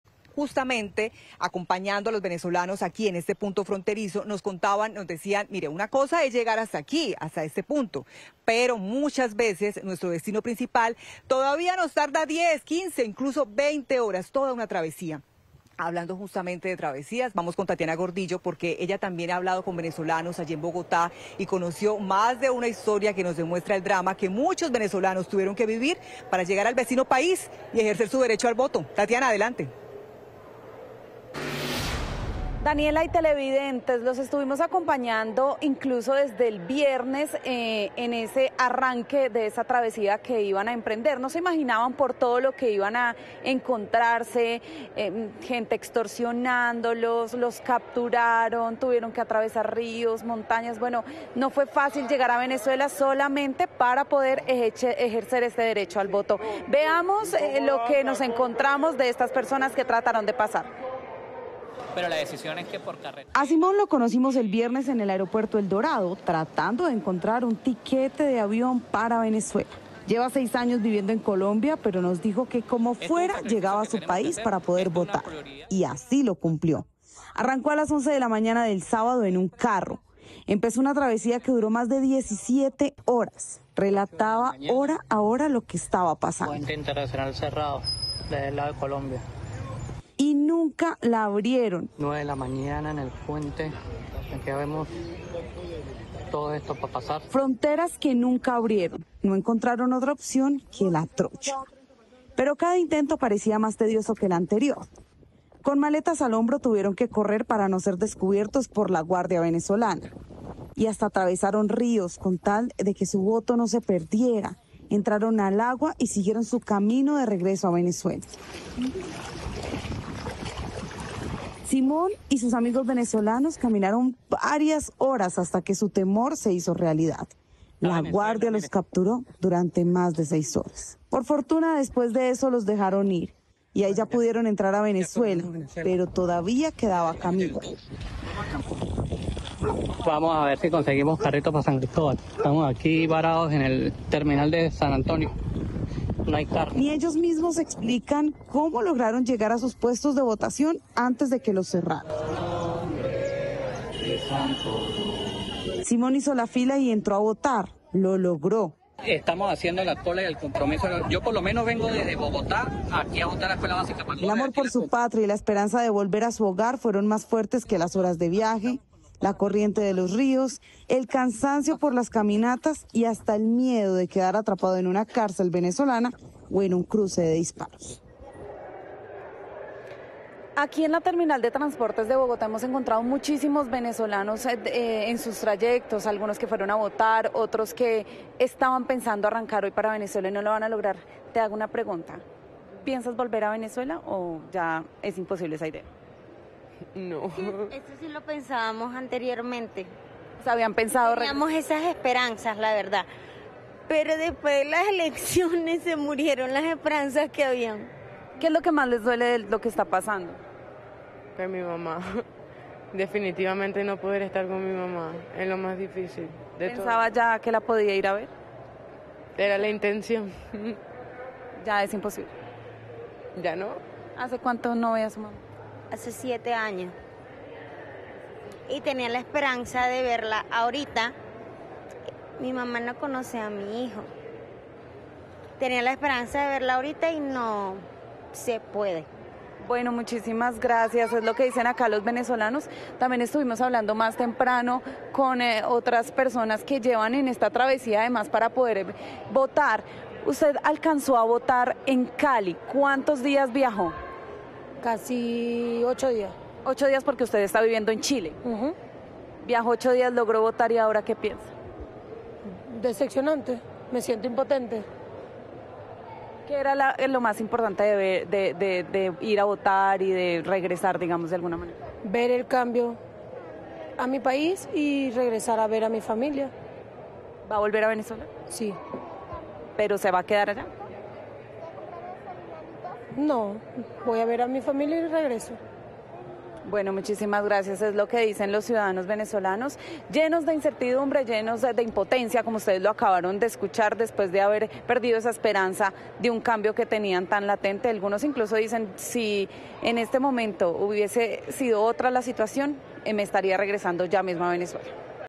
¿Cómo fue la travesía de venezolanos migrantes para poder votar?
Pese a que había un ambiente enrarecido y sospechas de fraude, cientos de venezolanos dejaron Colombia para ir a participar de las presidenciales. Esta es la historia de uno de ellos que se vio obligado a cruzar trochas y ríos para cumplir su cita con la democracia.

Noticias Caracol conoció la experiencia de Simón Gamboa, quien dijo que como fuera iba a llegar a su país y lo cumplió. El sábado 27 de julio arrancó su travesía a bordo de un carro y tras 17 horas llegó a la frontera con Cúcuta.